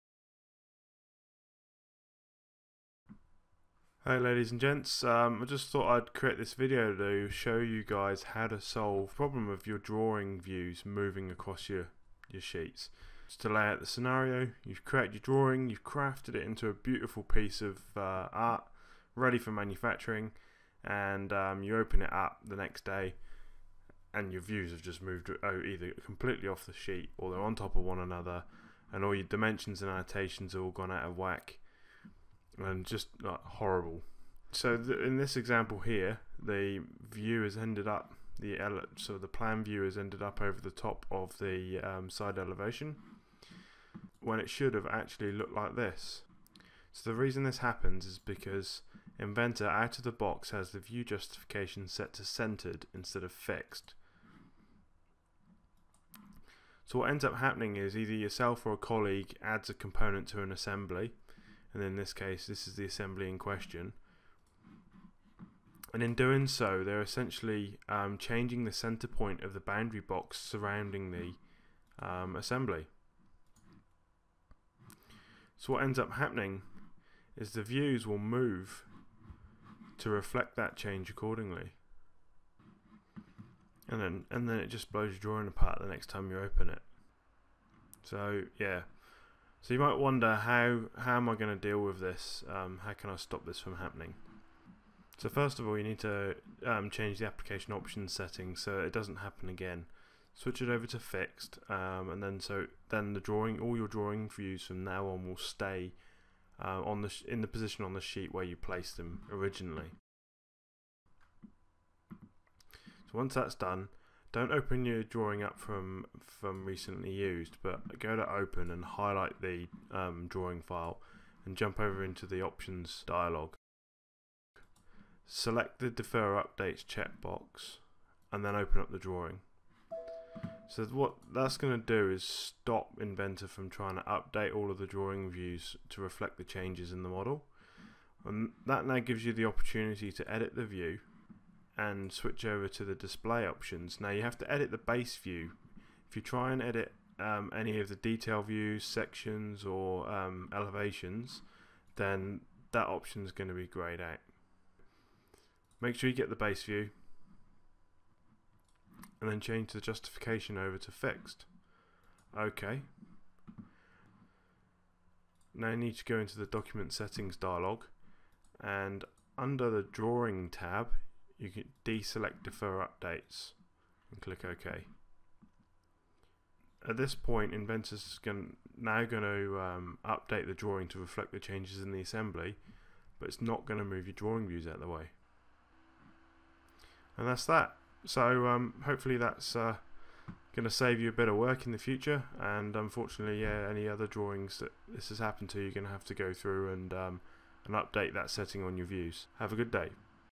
How to fix stray drawing views in Autodesk Inventor
You've crafted a beautiful drawing of an assembly, then the next day you come into work, and open it up to print for the lads (or gals) in the shop. Only you find all your drawing views have gone for a wander across your sheet, at this stage, you're not best pleased right? Well fret not, here is a way to stop it from happening again AND a method to fix your current drawing.